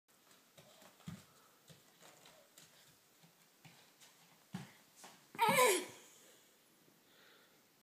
Elias går
Efter träning under midsommarfirandet på Härmanö så går vi nu...